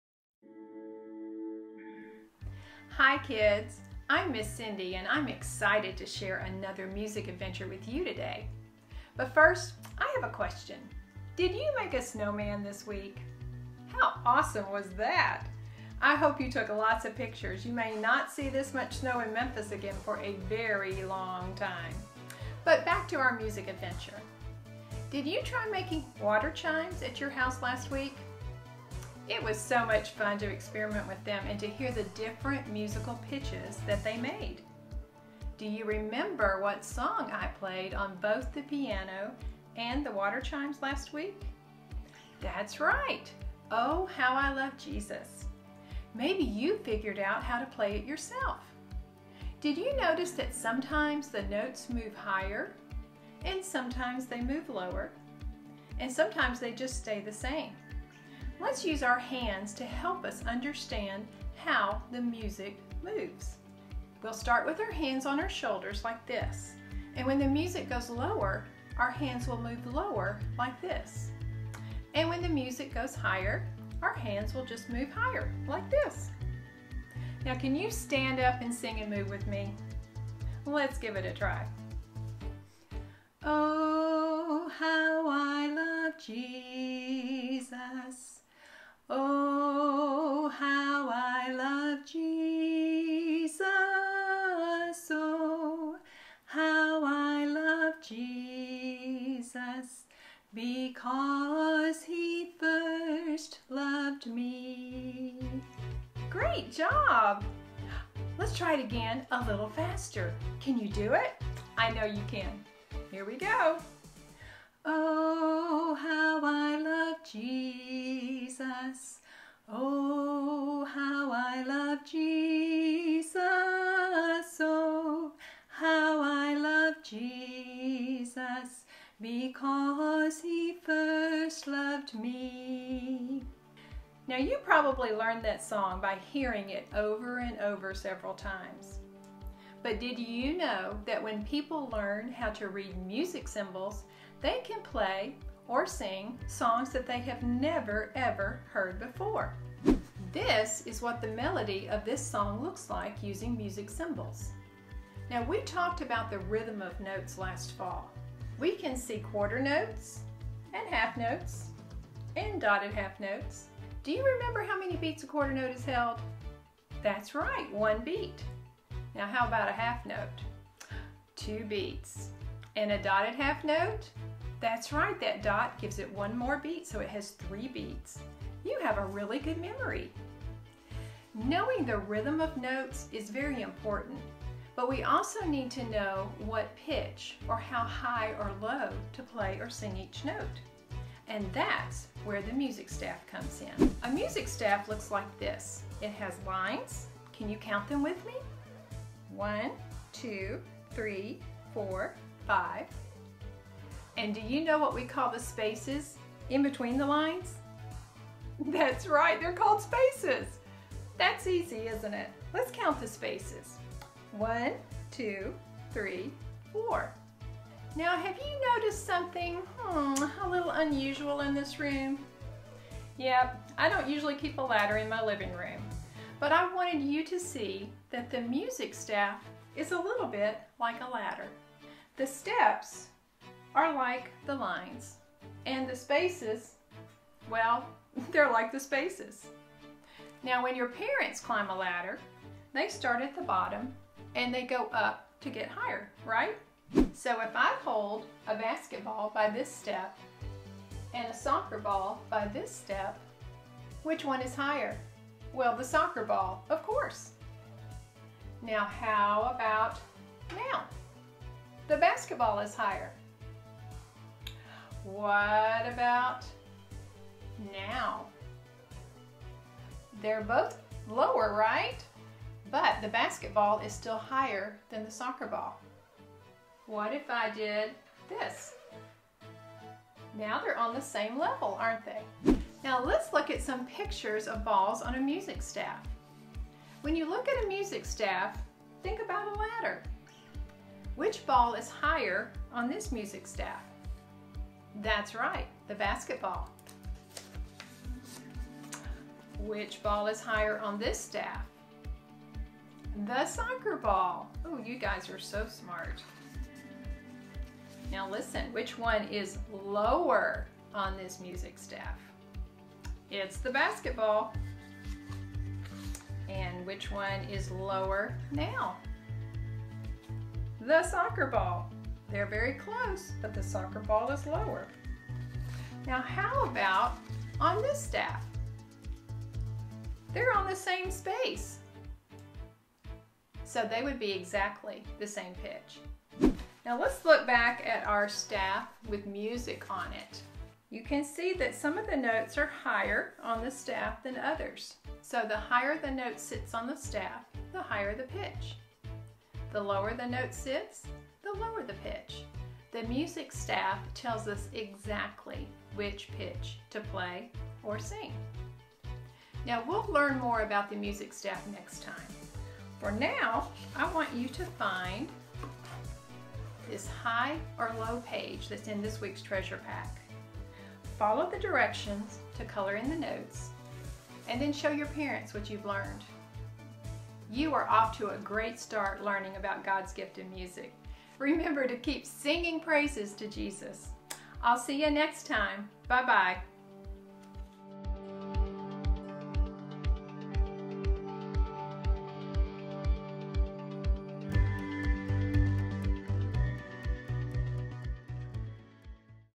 Ups and Downs of Music
Mrs. Cindy explains how pitch works on a music staff using a ladder and some balls!